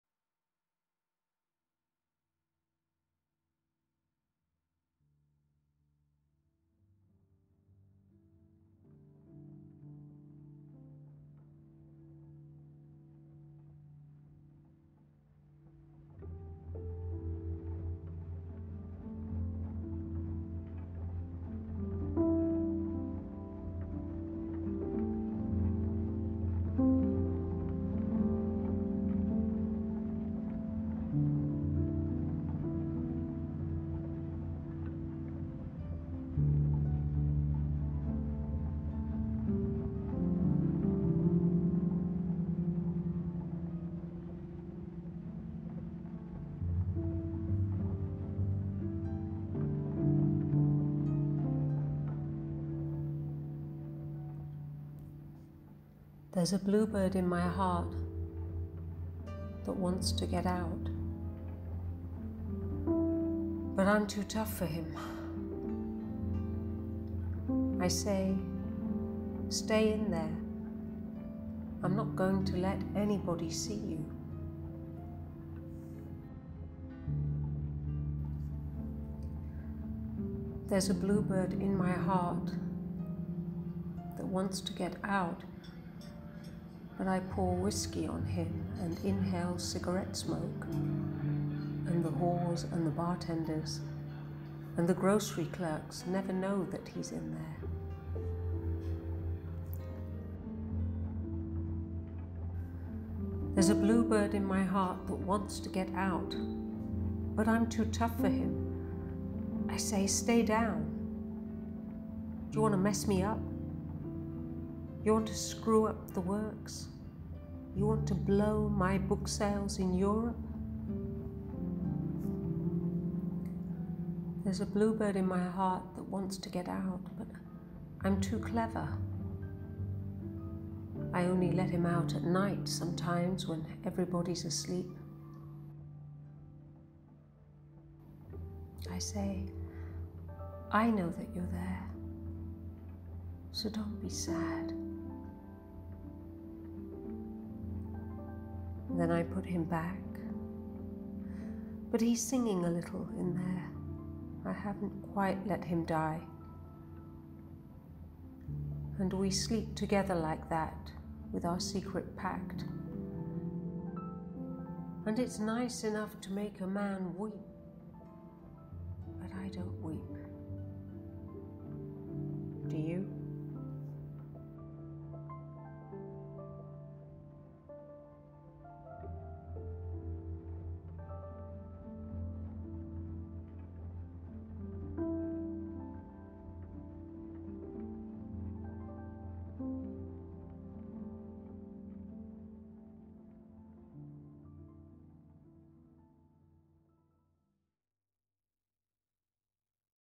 BLUEBIRD by Charles Bukowski
The story is one of love, but a love that had to remain secret for reasons that only later became known to me. I was commissioned to do the painting of the bluebird and the man with the blue eyes and they both feature in this video.